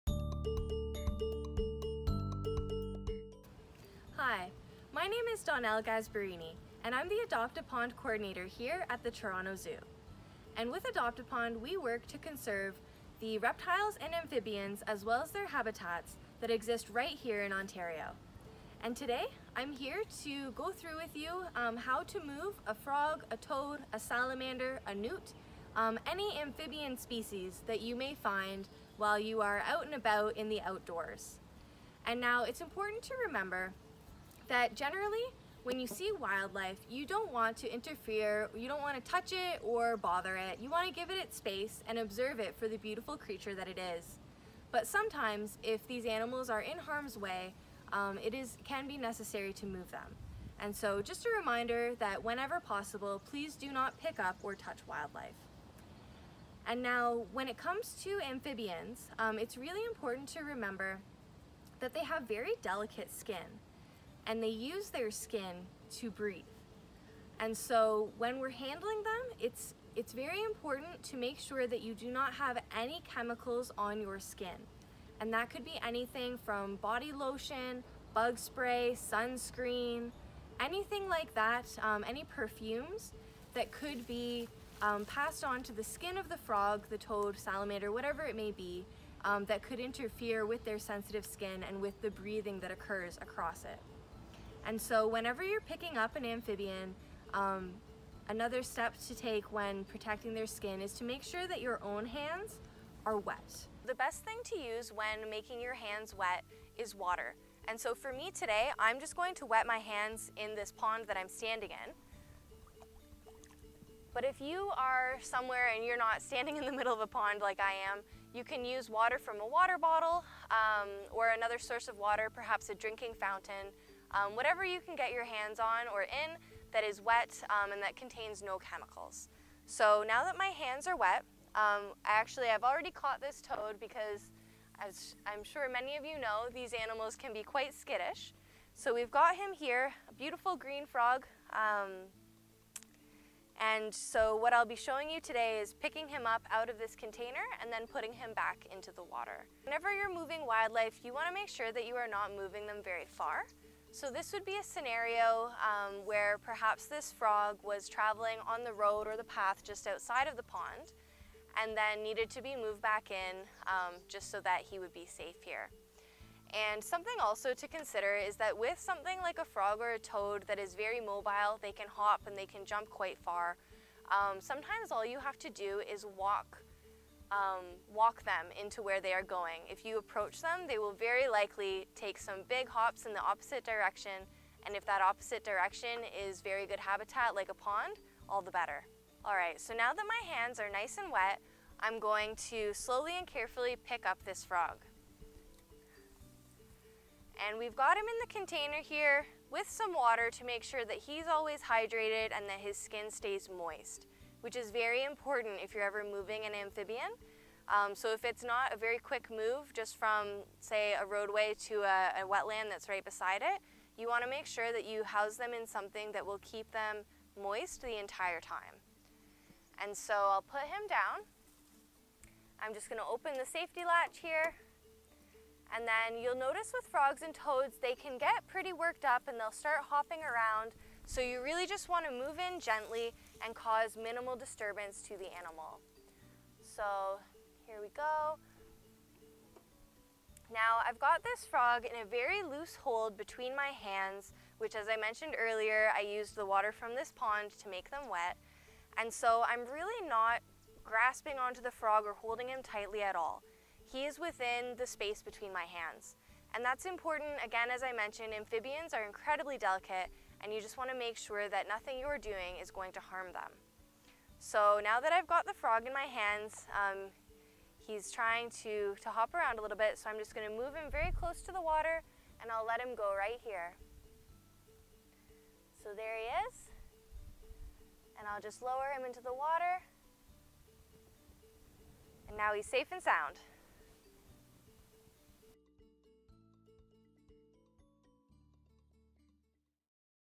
How To Move Amphibians Safely
Watch to learn how you can help frogs, toads, newts, and salamanders return to their wetland homes safely 🐸🦎